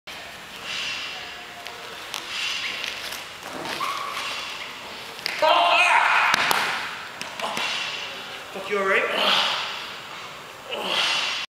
Armbreak ..Listen for the pop Brampton MMA fighter Pain Peters
No gi judo throws montage.no gi judo  Submission wrestling no gi judo with Pain Peters working some take downs n submission nhb mma style Brampton